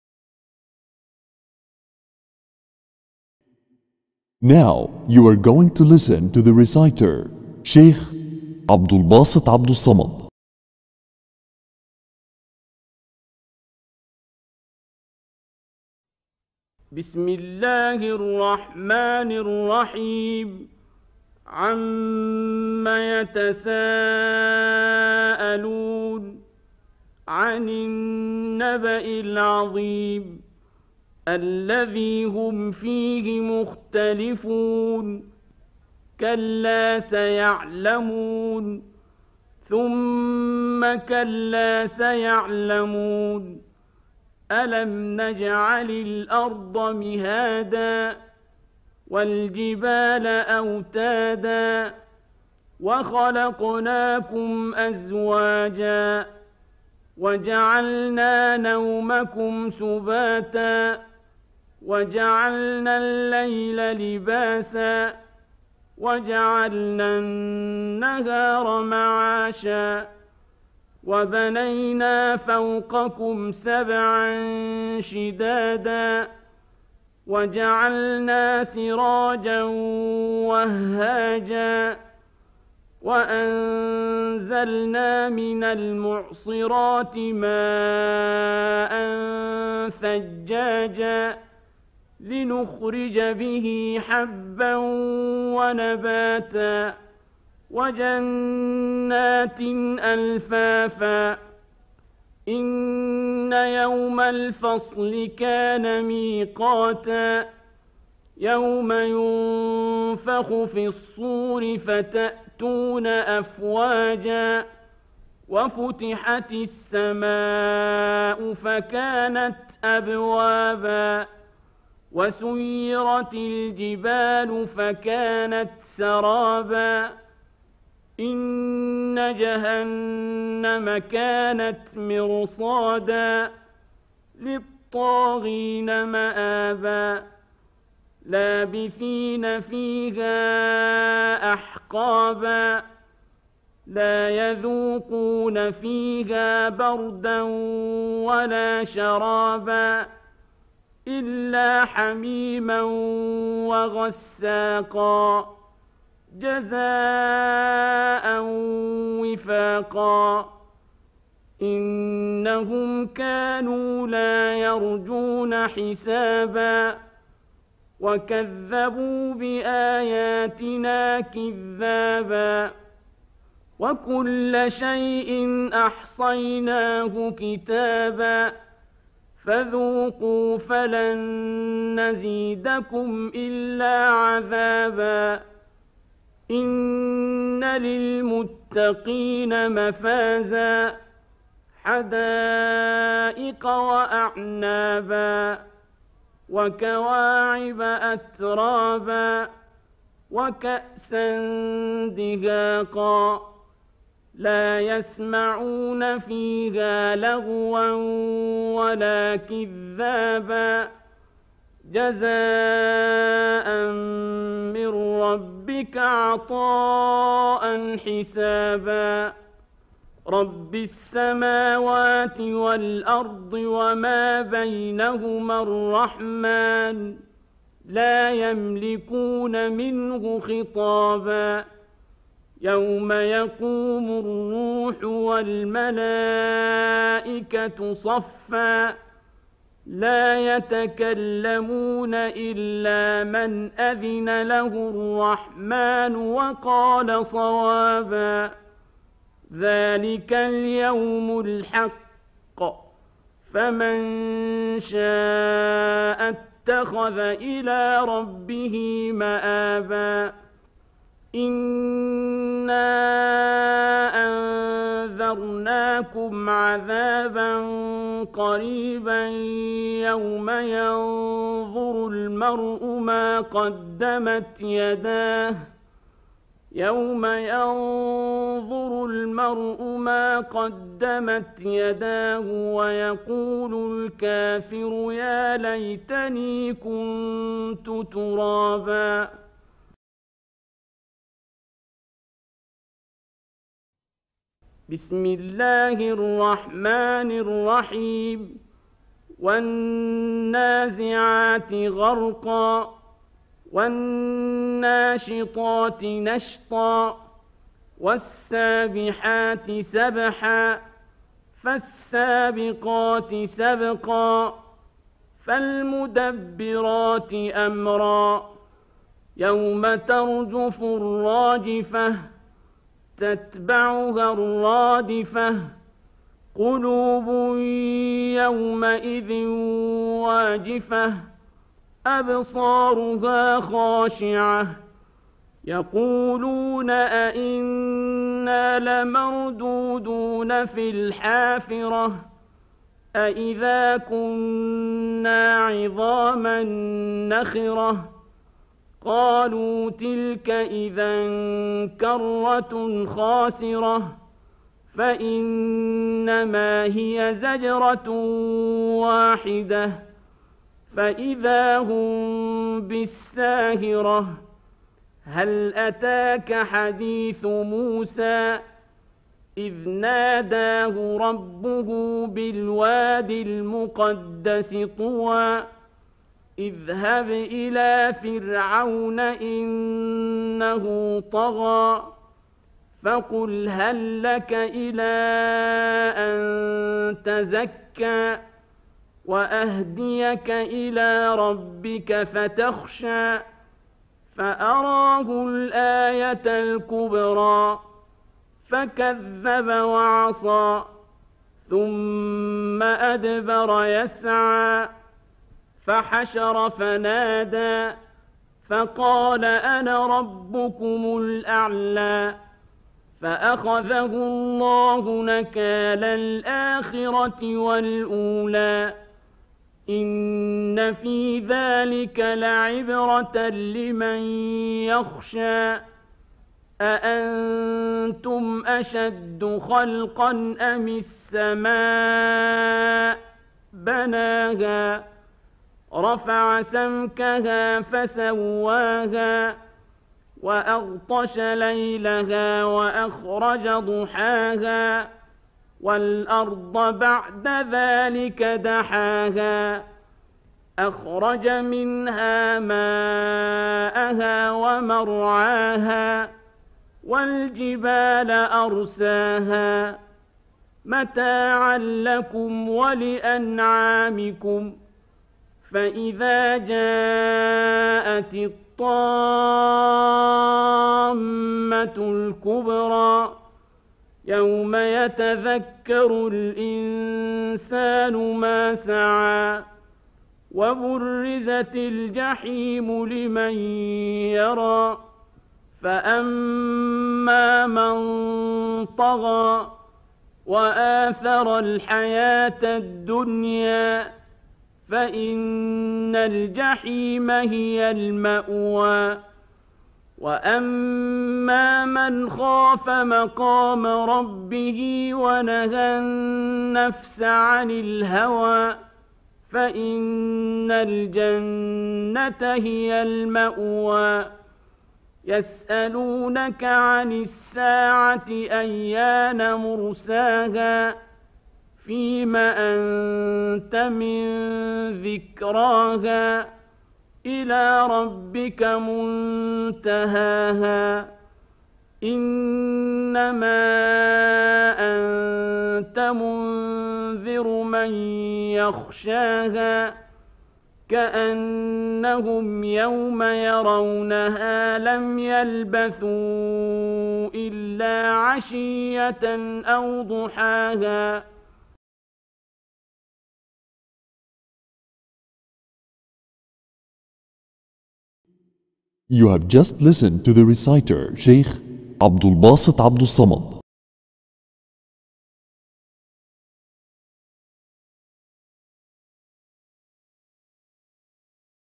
Surah Al Naba'a and Surah Al Nazahat
تلاوة سورة النبأ والنازعات كاملةً، مع ترجمة معانيها إلى اللغة الإنجليزية.